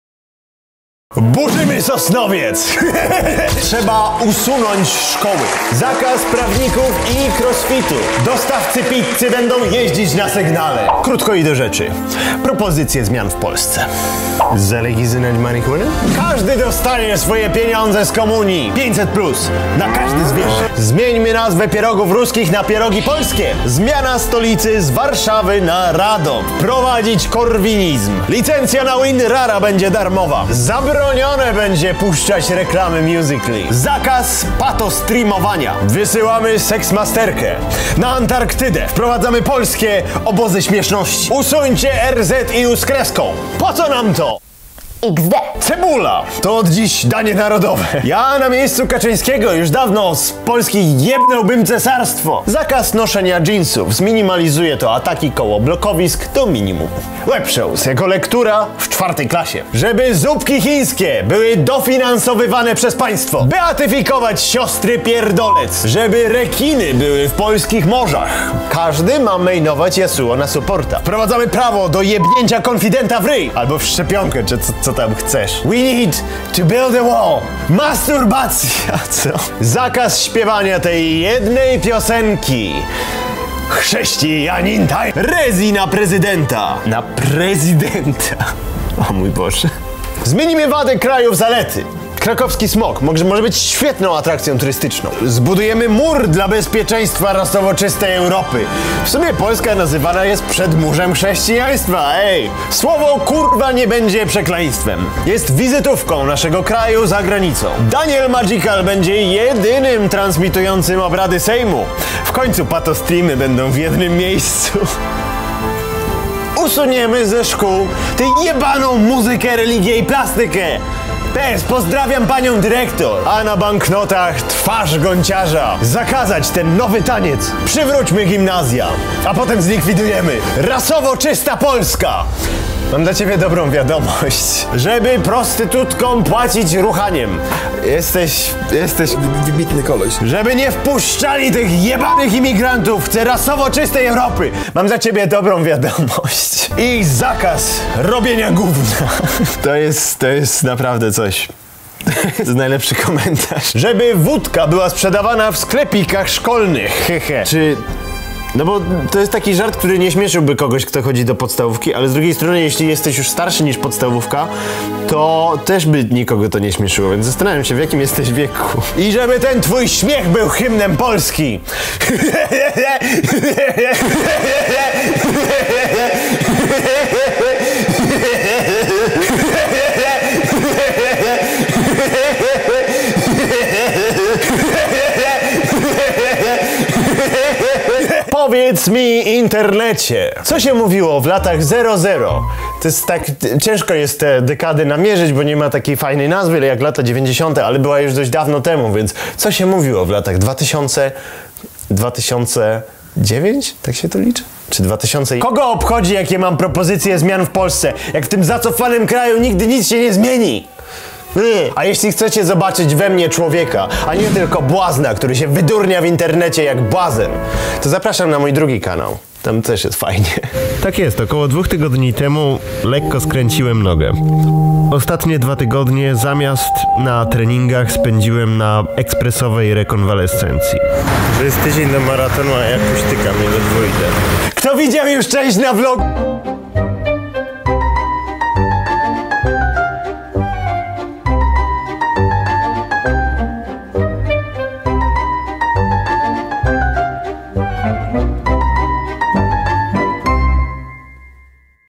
JAK NAPRAWIĆ POLSKĘ?
ⓘ Mądrość Tłumów - Internauci prezentują swoje plany reformy Polski!